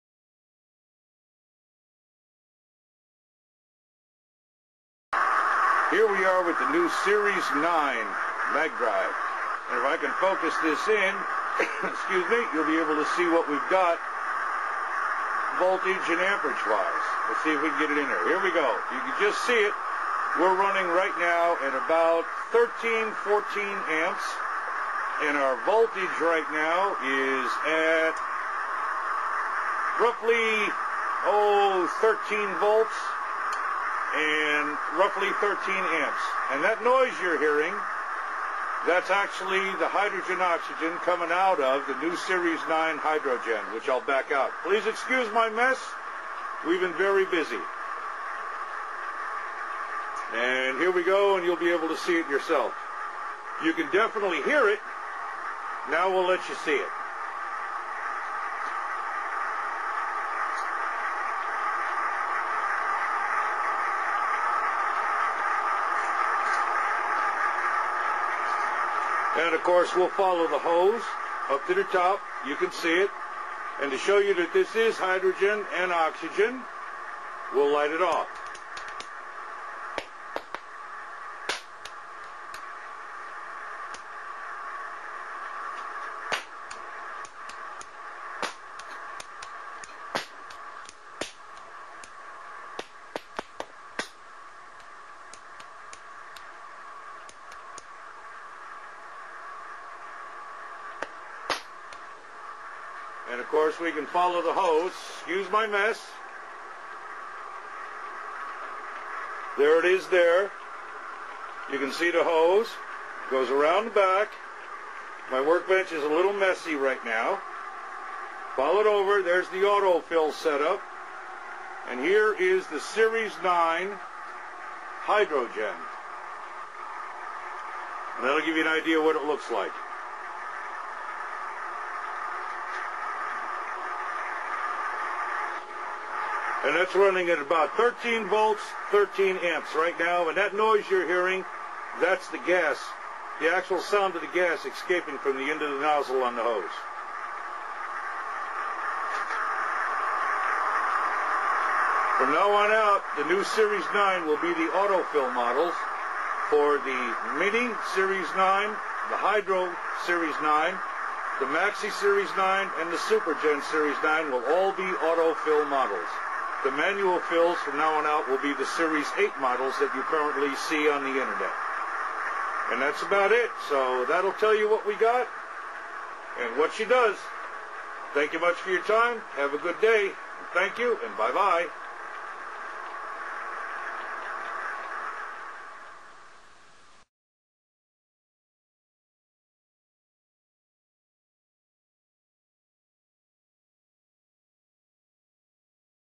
New Series9 Auto-Fill Hydro-Gen
Now half the size of the Series8 and double the HHO output using 12-13vlts DC and only 13 amps as opposed to the Series8 which ran on 12-13 vlts DC and 20-25amps. Considerably more HHO generation in a much smaller package.